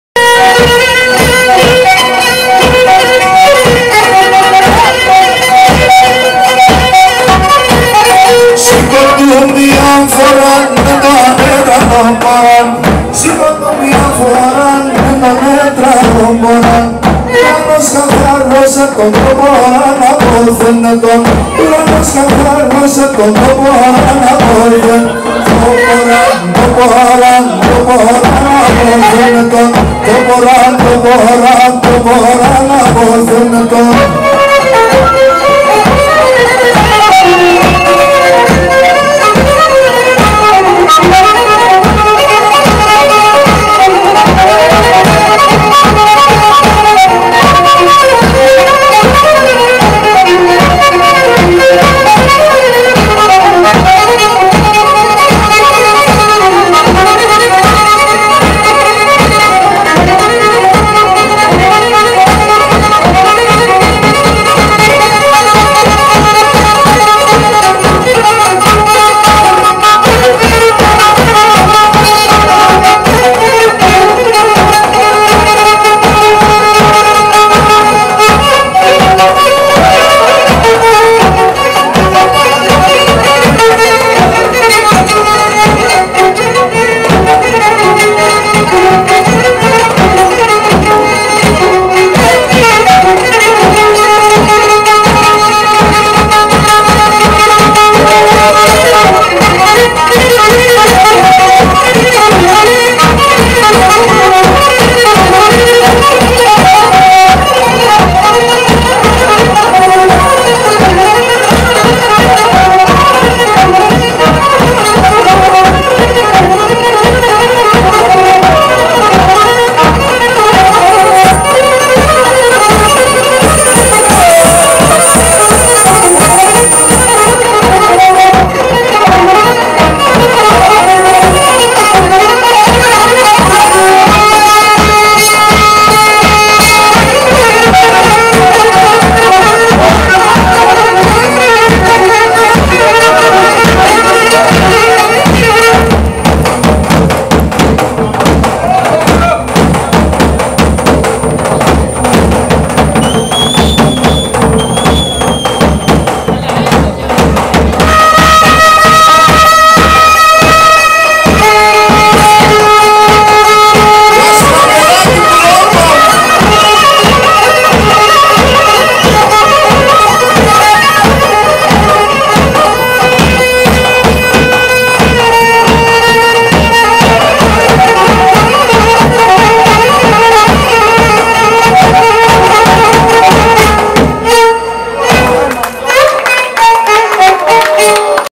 NTAOULI SOLO - Xoros Berolino - PART 5 - Panidis & Xalkidis & Spiridis [08.11.2008]
Pontiakos xoros sto Berolino stis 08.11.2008, meta to Synapantima.

Tragoudi: Kostas Panidis
Lyra: Sakis Xalkidis
Ntaouli: Vaggelis Spiridis